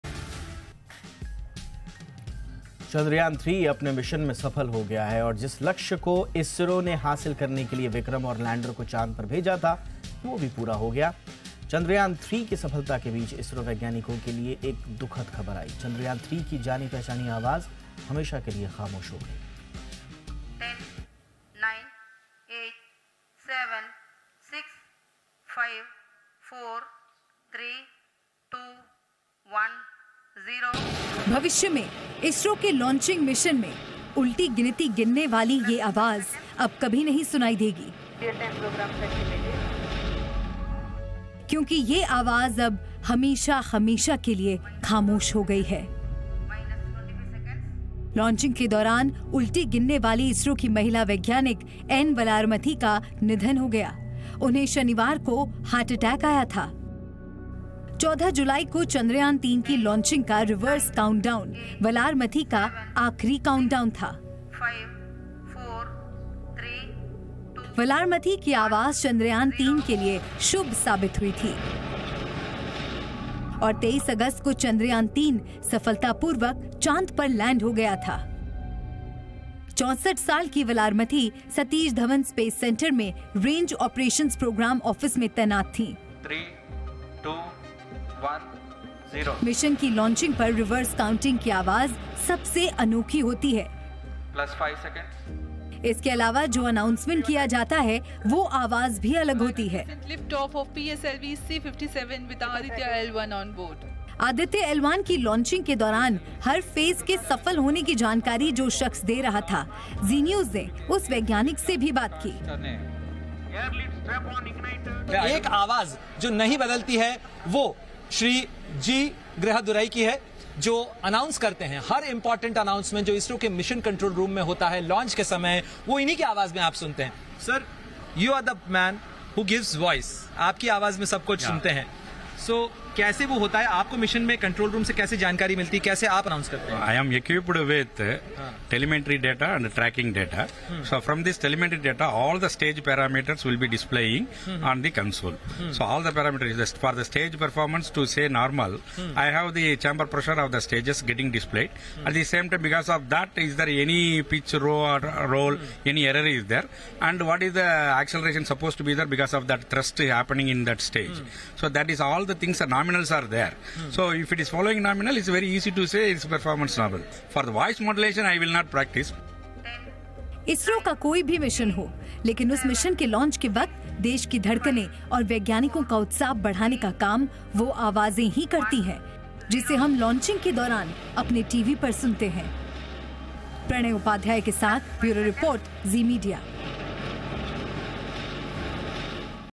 ISRO scientist N Valarmathi, voice behind Chandrayaan-3 countdown, dies
भारत के मून मिशन यानी चंद्रयान-3 के काउंटडाउन की गिनती करने वाली ISRO वैज्ञानिक वलारमथी का हार्ट अटैक के कारण निधन हो गया।